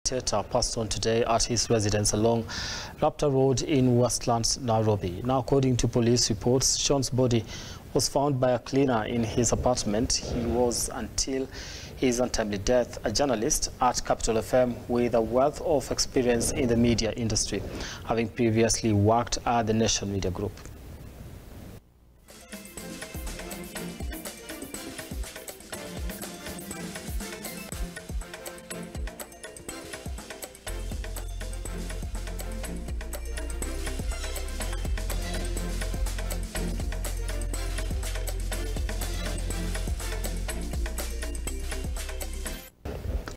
Sean Was Found Dead Today At His Residence In Westlands, Nairobi
Sean Cardovillis A Renowned Sports Commentator Passed On Today At His Residence Along Rhapta Road In Westlands, Nairobi.